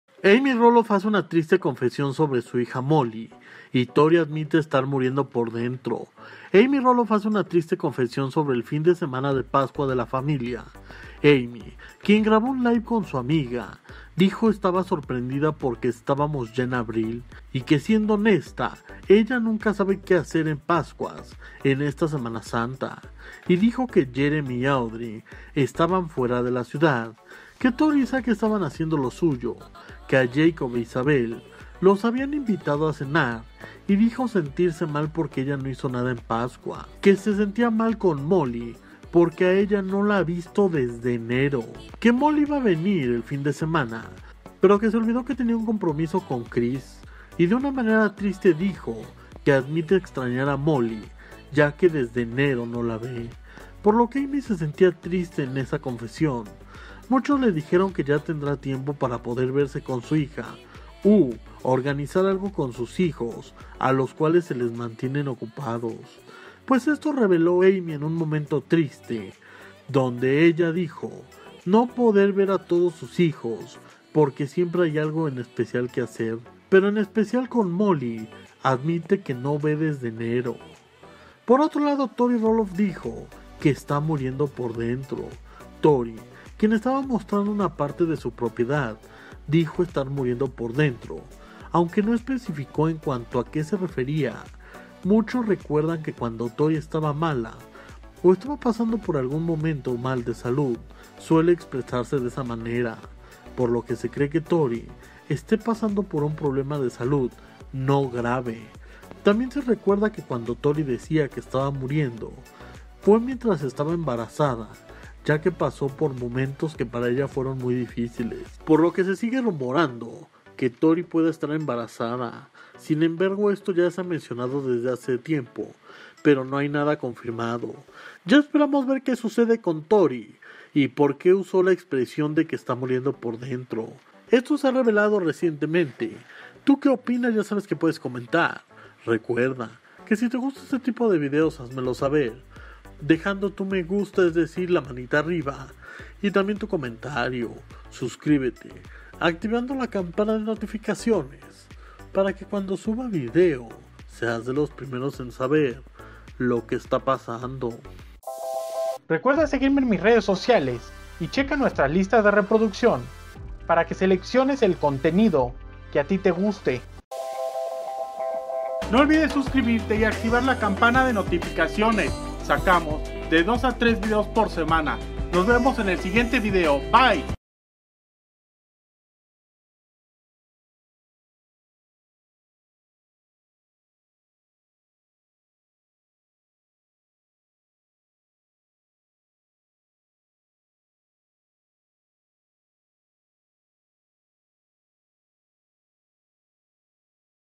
AMY ROLOFF HACE UNA TRISTE CONFESIÓN SOBRE MOLLY, TORI ADMITE ESTAR MURIENDO POR DENTRO
Xalapa Veracruz.